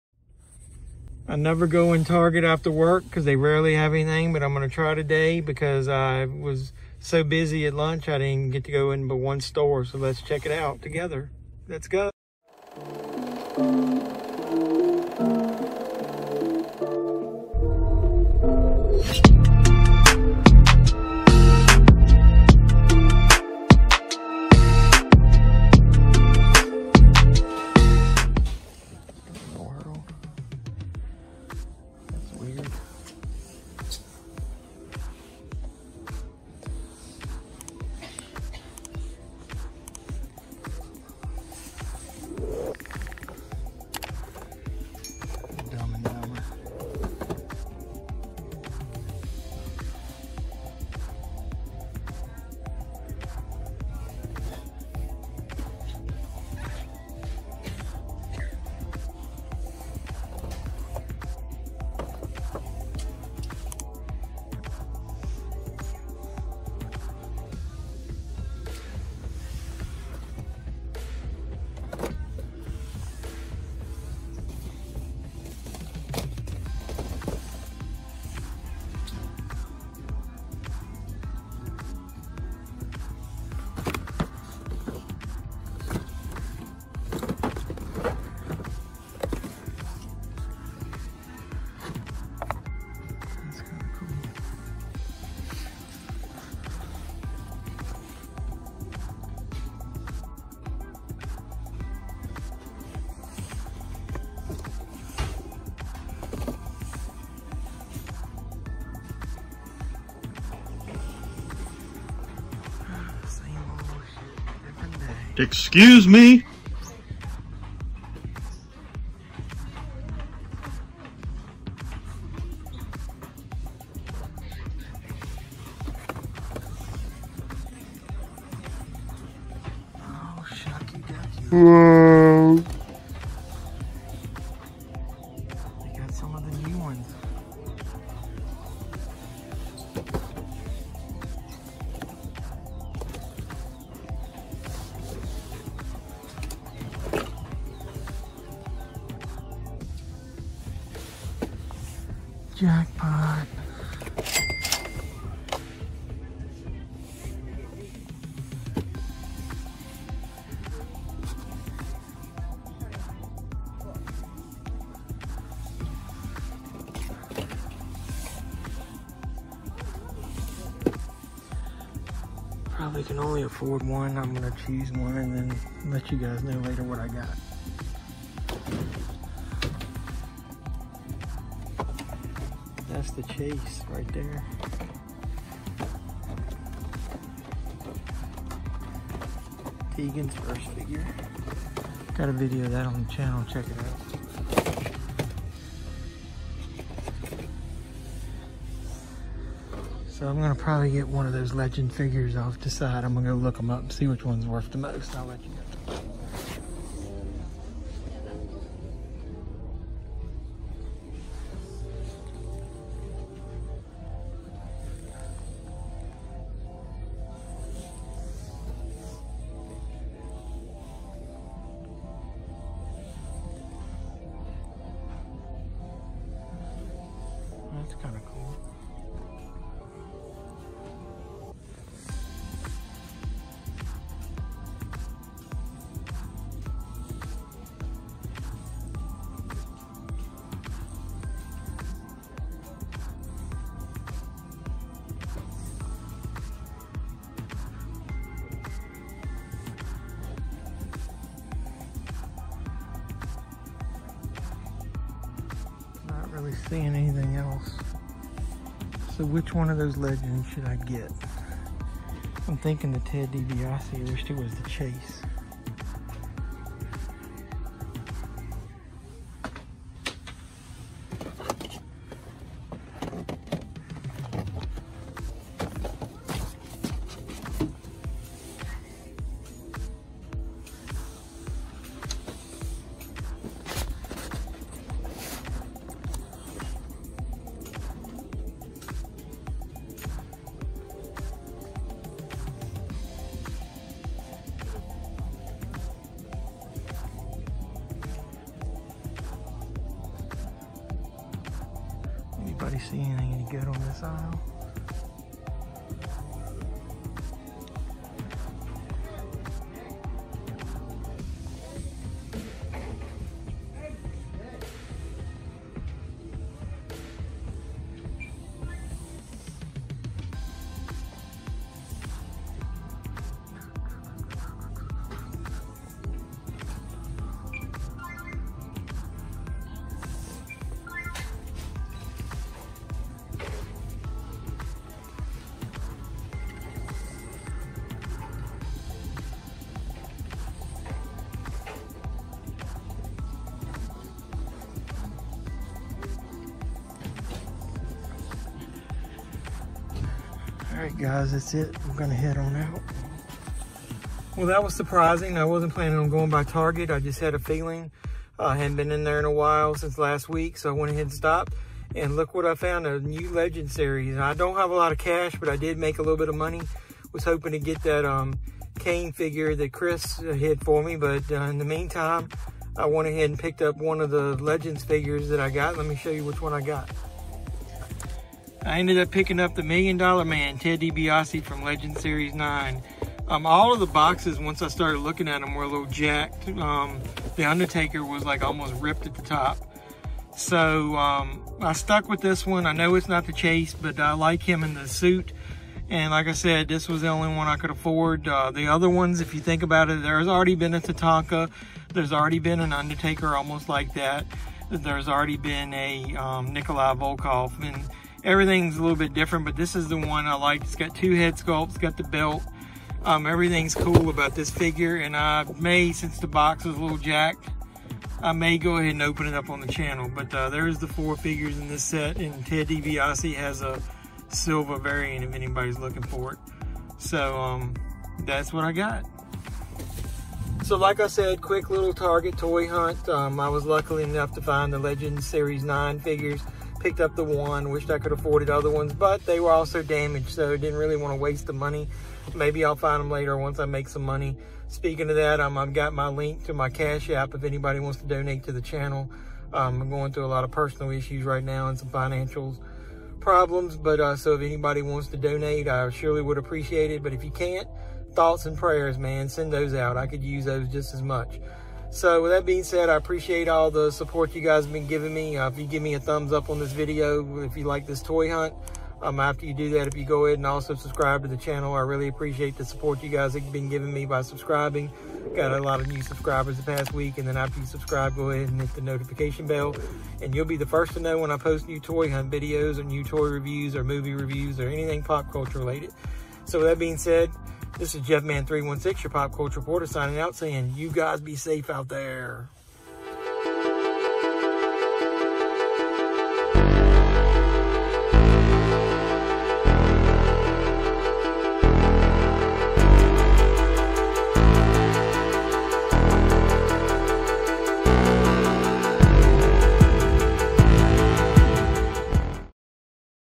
TOY HUNT #6 at Target - Searching for WWE Mattel Legends Series 9 Target Exclusives AEW
This is my 6th toy hunt video.  This contains a Target hunt for the new WWE Legends Series 9 figures which are exclusive to Target.  Did I find anything new or was the hunt hot garbage?  Join me and find out.

I may not be able to buy much because of some major personal issues, but I will still continue to search out toy deals and good figures for the hunt videos.